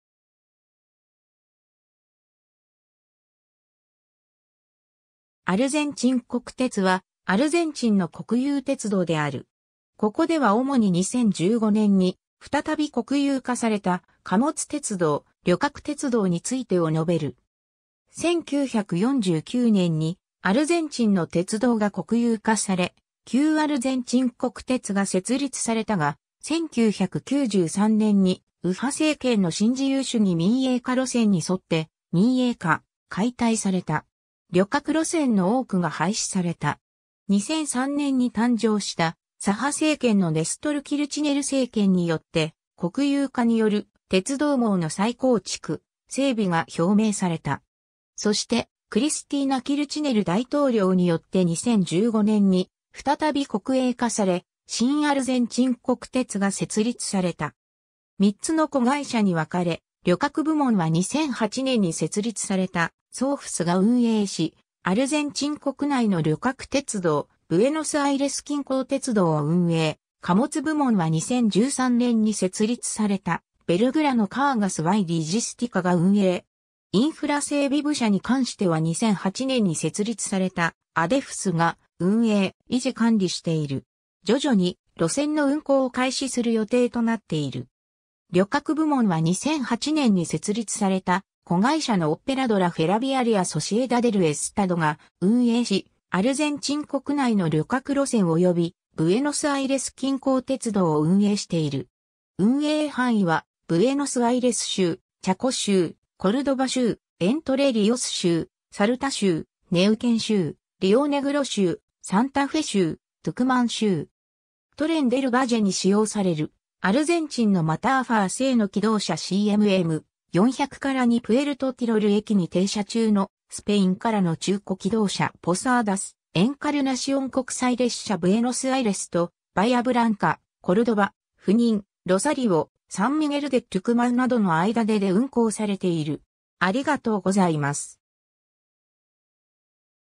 アルゼンチン国鉄
アルゼンチン国鉄（アルゼンチンこくてつ、スペイン語: Ferrocarriles Argentinos）は、アルゼンチンの国有鉄道である。
ここでは主に2015年に再び国有化された貨物鉄道、旅客鉄道についてを述べる。
 1949年にアルゼンチンの鉄道が国有化され、旧アルゼンチン国鉄が設立されたが、1993年に右派政権の新自由主義民営化路線にそって民営化・解体された。
旅客路線の多くが廃止された。
2003年に誕生した左派政権のネストル・キルチネル政権によって、国有化による鉄道網の再構築・整備が表明された。
そして、クリスティーナ・キルチネル大統領によって2015年に再び国営化され、新アルゼンチン国鉄が設立された。
3つの子会社に分かれ、旅客部門は 2008年に設立されたSOFSEが運営し、アルゼンチン国内の旅客鉄道、ブエノスアイレス近郊鉄道を運営、貨物部門は2013年に設立されたBelgrano Cargas y Logísticaが運営。
インフラ整備部者に関しては2008年に設立されたADIFSEが運営・維持管理している。
徐々に路線の運行を開始する予定となっている。
 旅客部門は2008年に設立された子会社のOperadora Ferroviaria Sociedad del Estado（SOFSE）（通称:Trenes Argentinos）が運営し、アルゼンチン国内の旅客路線及びブエノスアイレス近郊鉄道を運営している。
運営範囲はブエノスアイレス州/チャコ州/コルドバ州/エントレ・リオス州/サルタ州/ネウケン州/リオネグロ州/サンタフェ州/トゥクマン州。
 Tren del Valleに使用されるアルゼンチンのMaterfer製の気動車"CMM 400-2" プエルトティロル駅に停車中のスペインからの中古気動車（SE.FE.CHA.時代、現在はMaterfer CMM 400-2を使用） ポサーダス - エンカルナシオン国際列車（"Wadloper"） ブエノスアイレスとバイアブランカ、コルドバ、フニン、ロサリオ、サン・ミゲル・デ・トゥクマンなどの間でで運行されている。